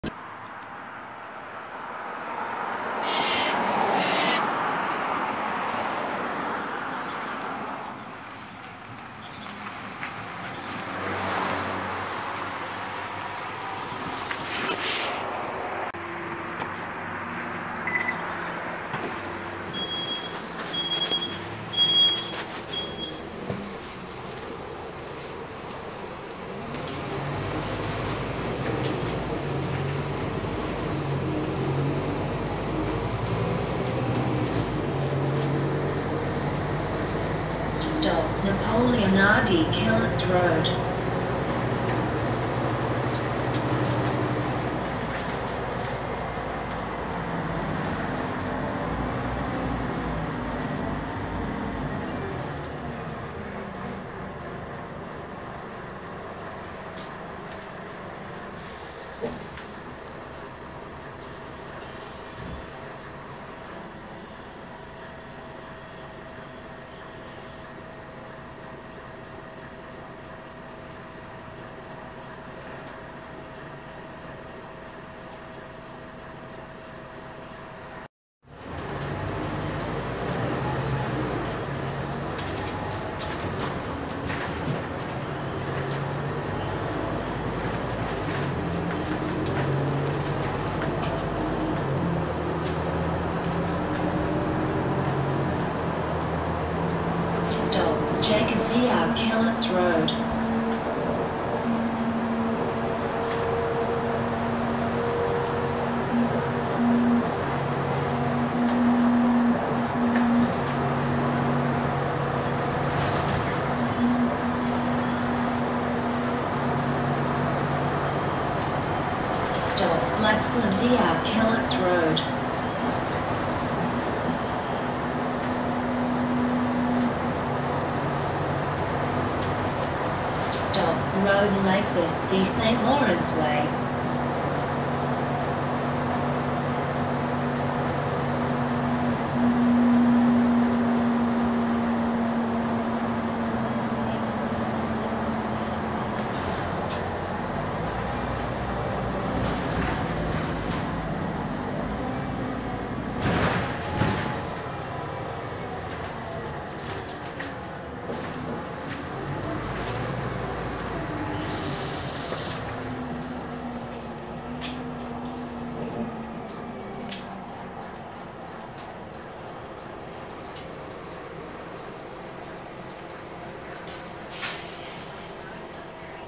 Random June 20th Filler Video.
A ride on route 691 from Lakesfield Dr/Napoleon Rd to Rowville Lakes SC on Ventura Bus 1524 from Knoxfield Depot.
Because Rowville so of course why not. Chances are, I'll record this again.